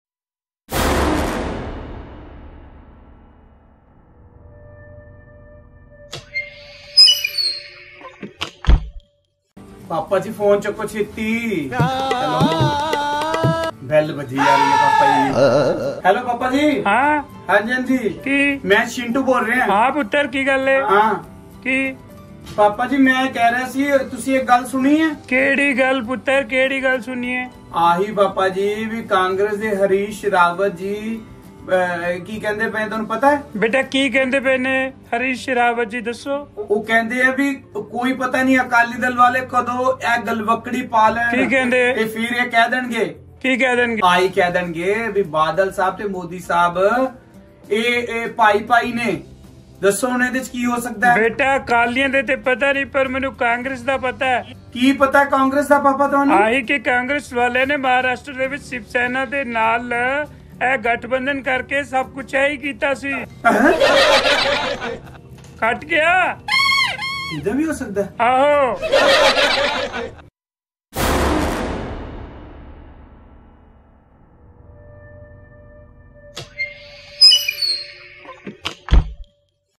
ਸੁਖਬੀਰ ਬਾਦਲ ਦਾ ਕੋਈ ਪਤਾ ਨਹੀਂ ਅਕਾਲੀ ਗਲ਼ਵੱਕਡ਼ੀ ਪਾ ਕੇ ਫਰਿ ਕਹਿ ਦੇਣ 'ਮੋਦੀ-ਬਾਦਲ ਭਾਈ-ਭਾਈ' | Punjabi News
ਸੁਖਬੀਰ ਬਾਦਲ ਦਾ ਕੋਈ ਪਤਾ ਨਹੀਂ ਅਕਾਲੀ ਗਲ਼ਵੱਕਡ਼ੀ ਪਾ ਕੇ ਫਰਿ ਕਹ ਿਦੇਣ 'ਮੋਦੀ-ਬਾਦਲ ਭਾਈ-ਭਾਈ': ਪੰਜਾਬ ਕਾਂਗਰਸ਼ Bhagwant maan challenge to sukhbir badal on agriculture bills 1